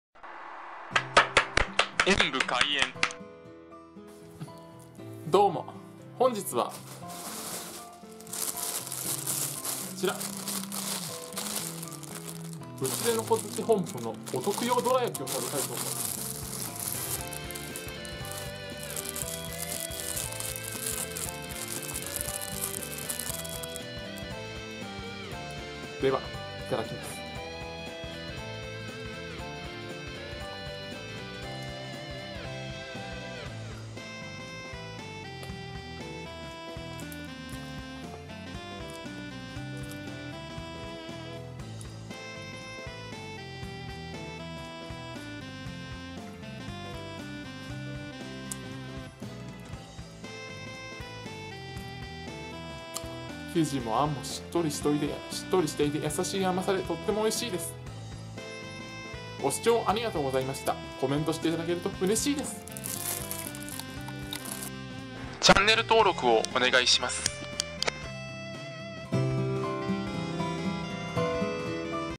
神戸堂 どら焼きを食べてみた
神戸道のどら焼きを食べました。
あっさりしていますね～